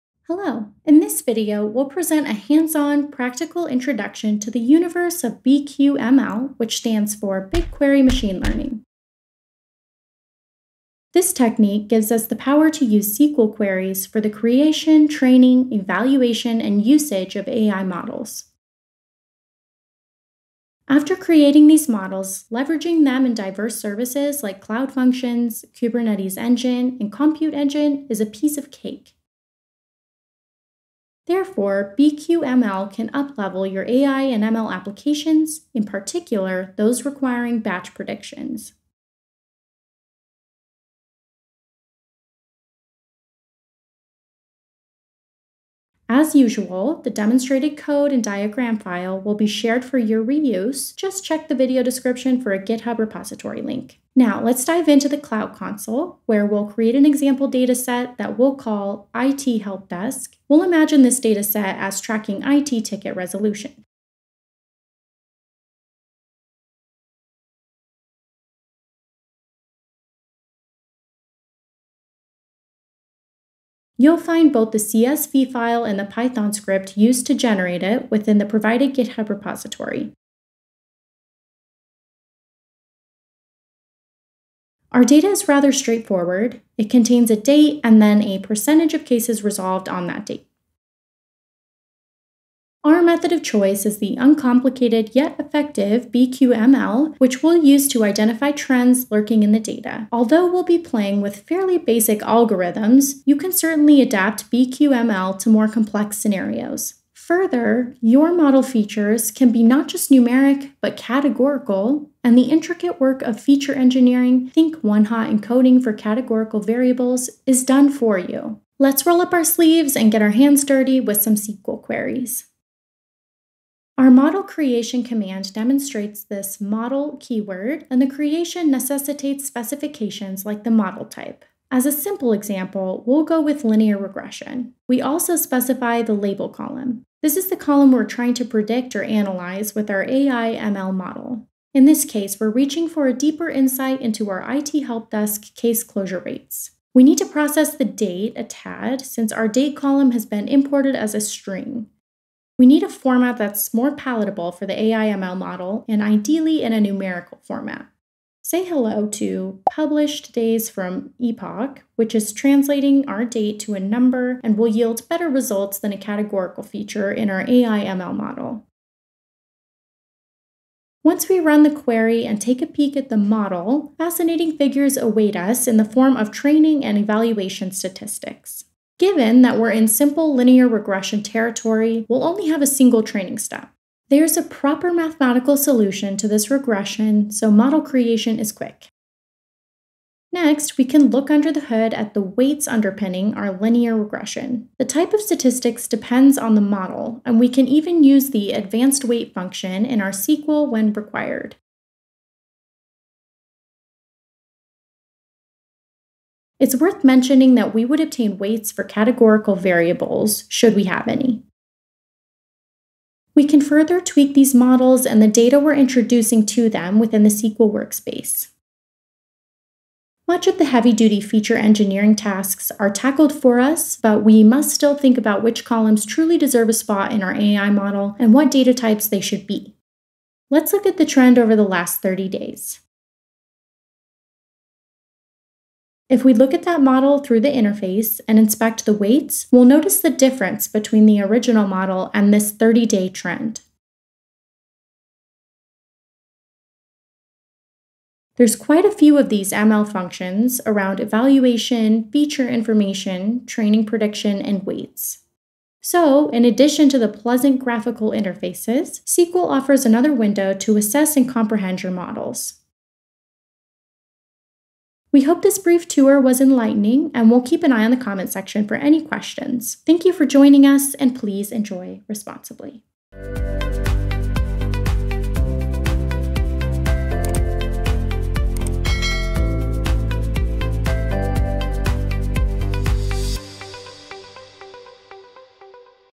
Hands-On Intro to BQML (BigQuery Machine Learning)
Welcome to our practical tutorial on Google's BigQuery Machine Learning (BQML). Learn how to create, train, and evaluate AI models using SQL queries.

0:00 Conceptual Overview
0:54 BigQuery Dataset
1:32 BigQuery ML (BQML)